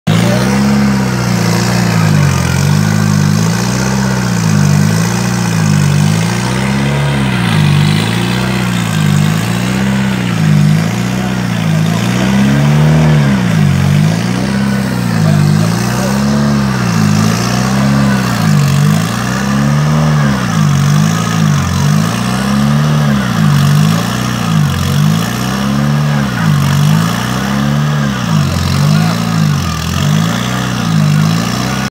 Swidnica. Rajd Swidnicki 2019- Brzezinski ,testy Ford Fiesta Proto.
Jakub Brzezinski i ostatnie testy przed startem do OS 1.